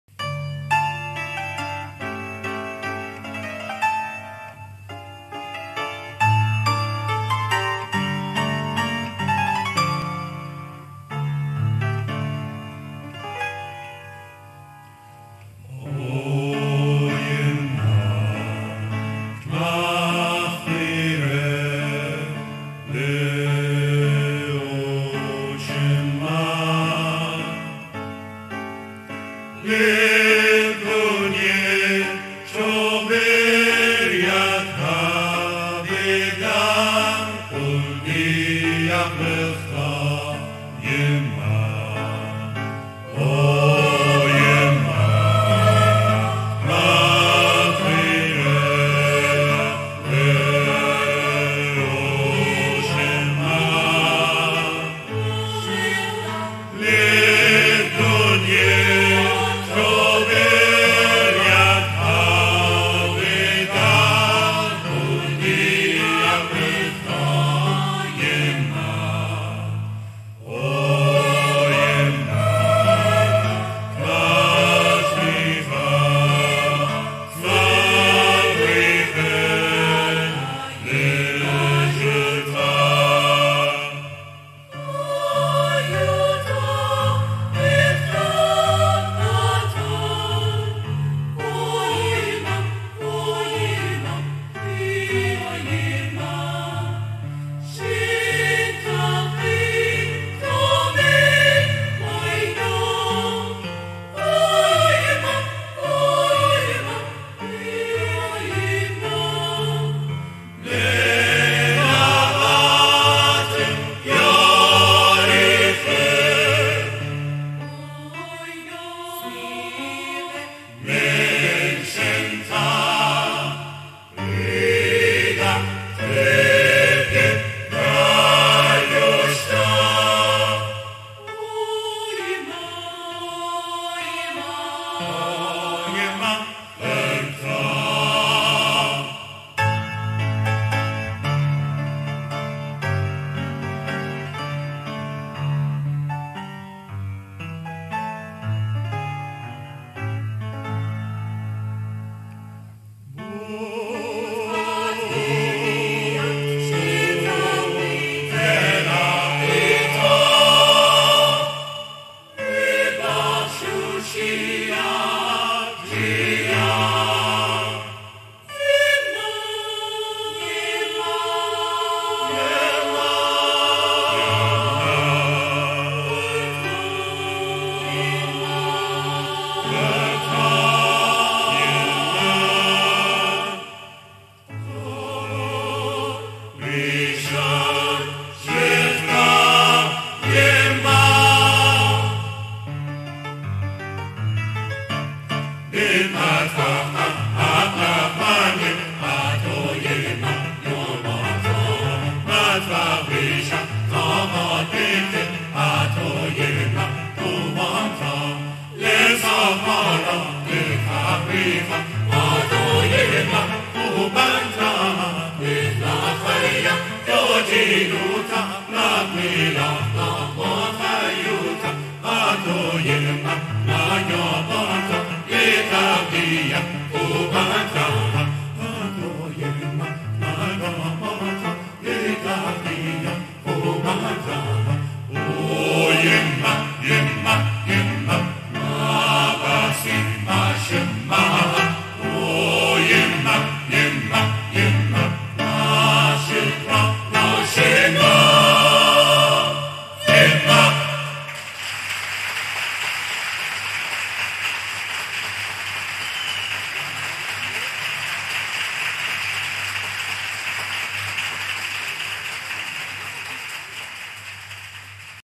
اجرای گروه همسرایان ملی آشوری به مناسبت عید میلاد مسیح و سال نو 2004 در سالن ویلیام دانیال- 6
Assyrian National Choir on the occasion of Christmas and New Year 2004 in Tehran William Daniel Hall in Tehran
اجرای زیبای گروه همسرایان ملی آشوری به مناسبت عید میلاد مسیح و سال نو 2004 در سالن ویلیام دانیال تهران